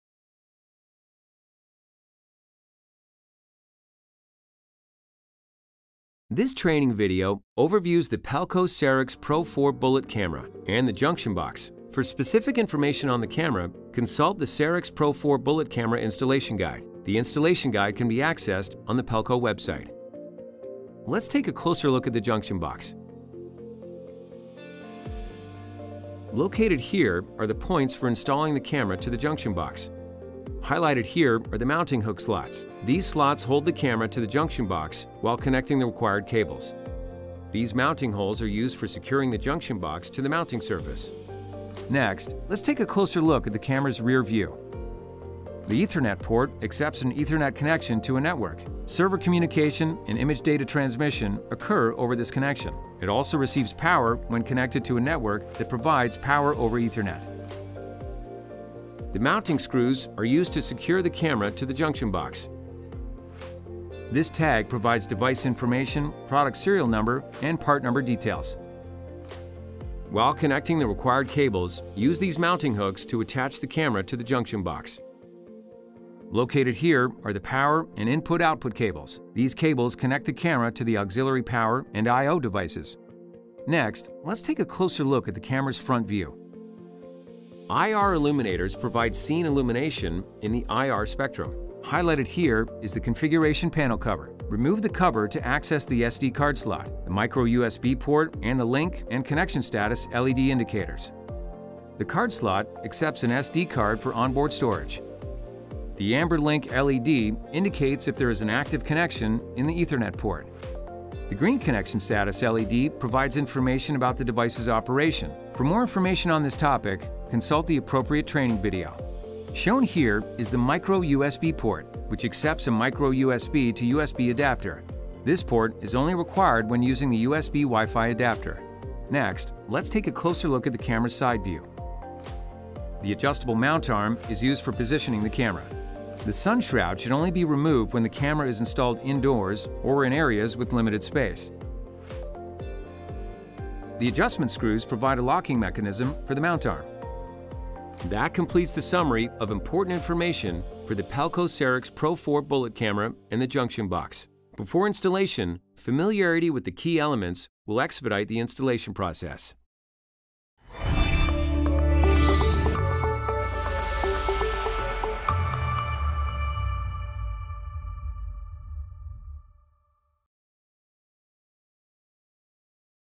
Sarix Professional 4 Bullet Camera Overview
This training video overviews the Pelco Sarix Professional 4 Bullet camera.